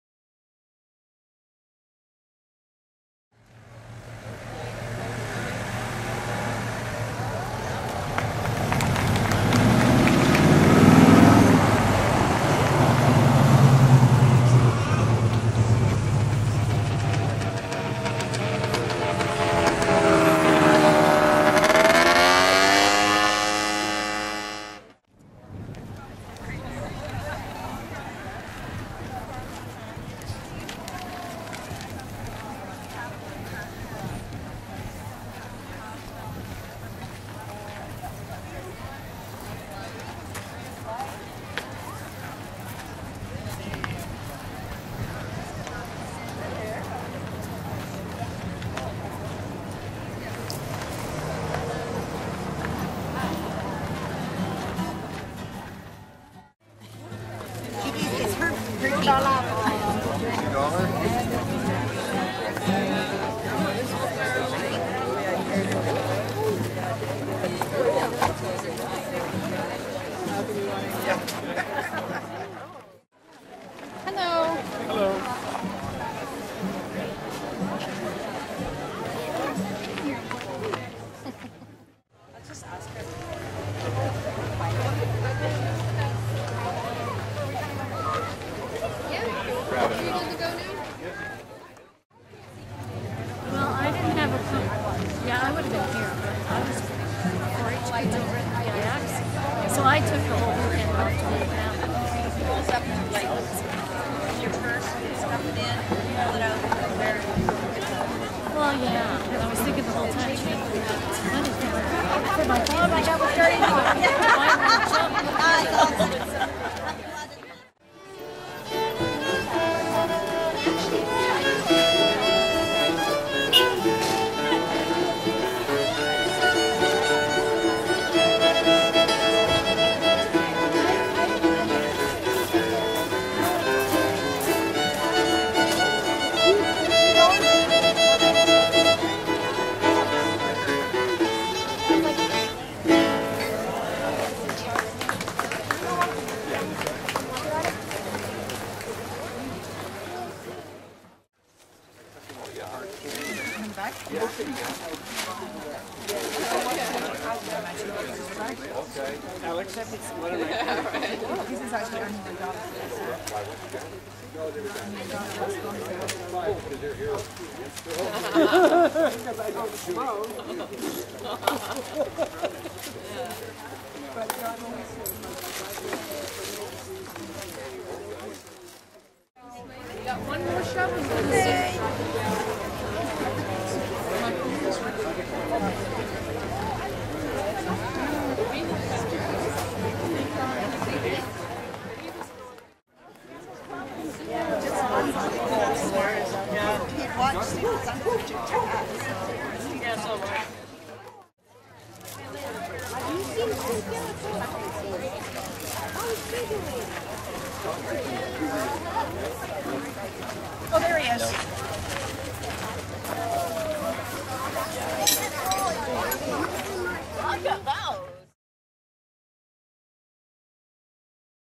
Gabriola Farmers Market
Gabriola Island Farmer's Market located at the Agi Hall on Saturday's through the summer from 9AM to 12PM.  The Market has local artists, local talent, local baking and treats, a great family shopping spree or just breakfast.